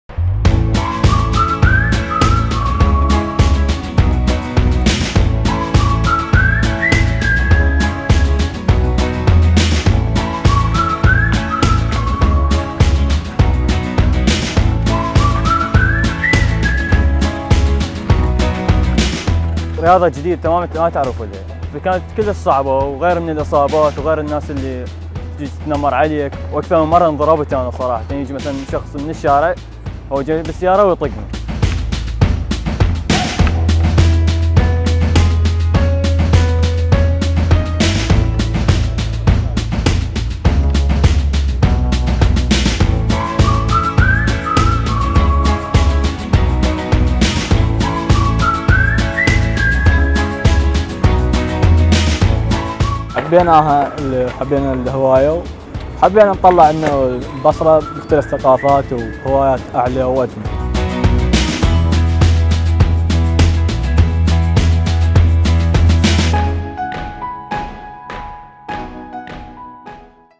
بعد تخفيف القيود.. التزلج في شوارع البصرة
بعد تخفيف قيود كورونا.. شباب البصرة العراقية يعيدون استكشاف مدينتهم بـ"التزلج"

ـــــــــــــــــــــــــــــــــــــــــــــــــــــــــــــــــــــــــــــــــــــــــــــــــــــــــــــــــــــــــــــــــــــ
تيلغرام
سناب شات alarabiya
تفاعلكم
صباح العربية
بانوراما
العربية الليلة
Live Alarabiya
Alarabiya Live